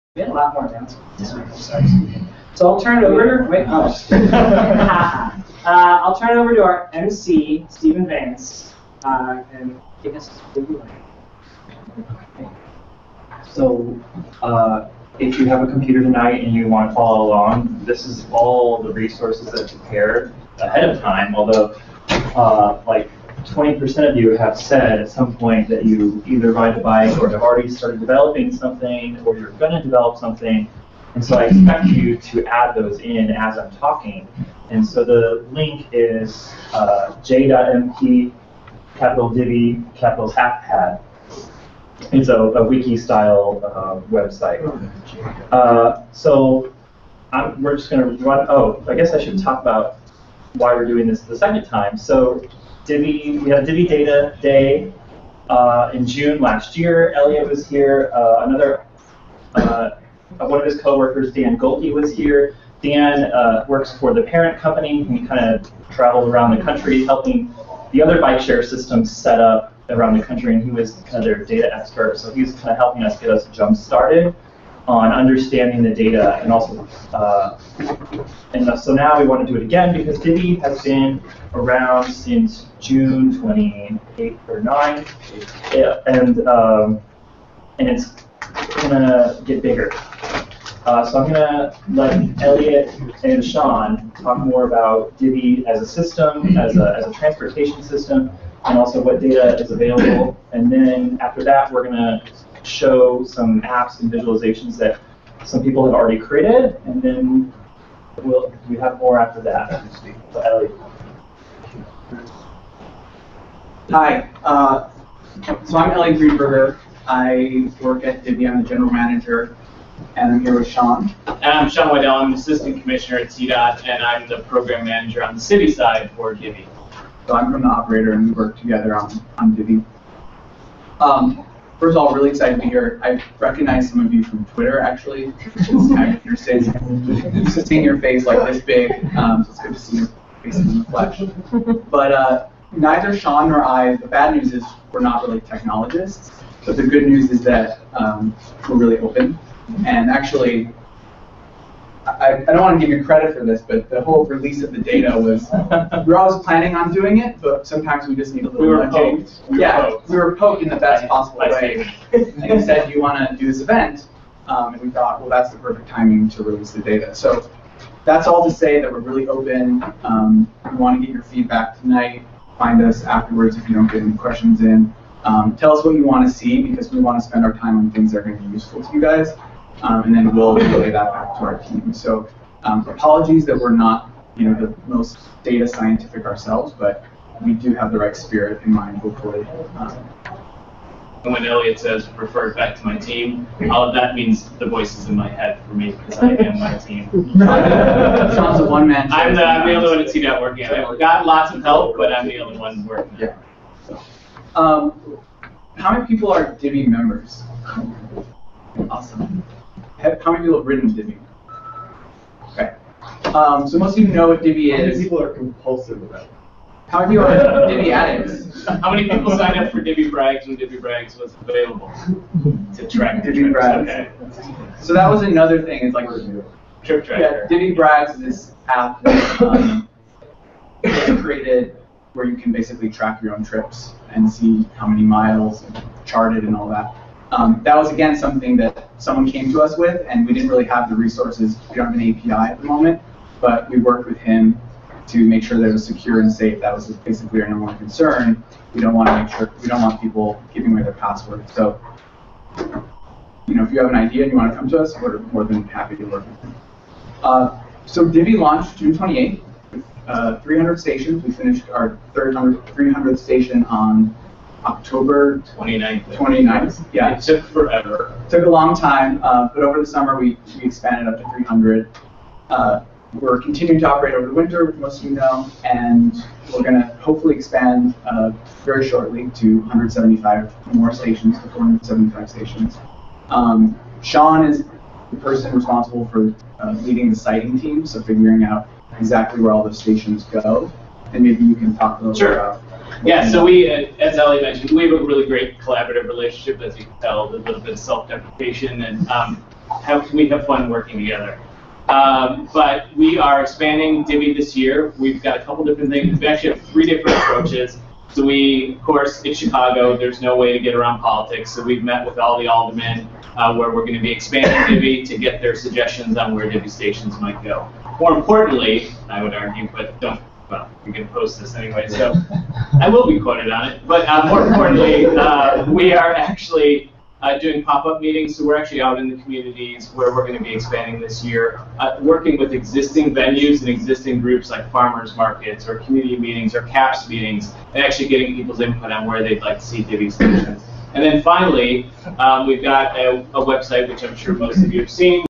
Divvy Data II: Part One About Divvy
Here's Elliot Greenberger from Divvy and Gabriel Gaster from the City of Chicago talking about the Divvy system.